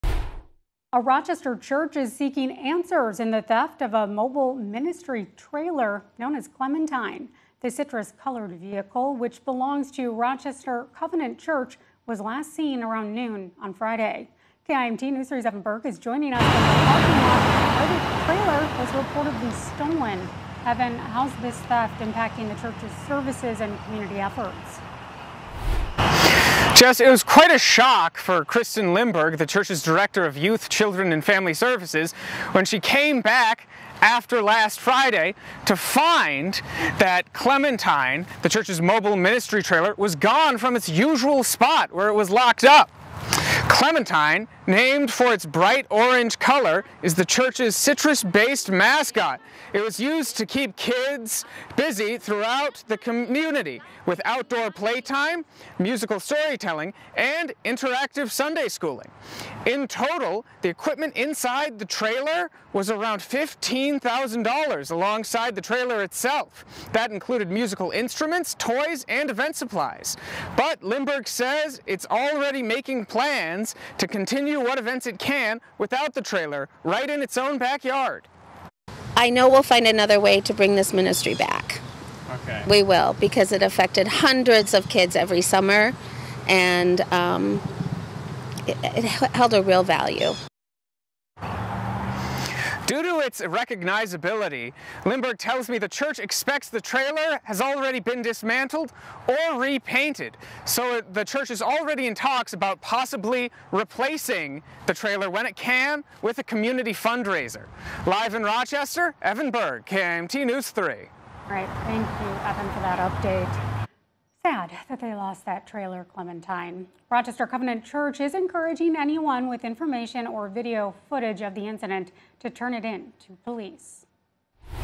Rochester Covenant Church adapts after trailer theft leaves it without outdoor services
KIMT News Three's Evan Berg spoke with the church about the impact this will have, not just on the congregation, but the community as a whole.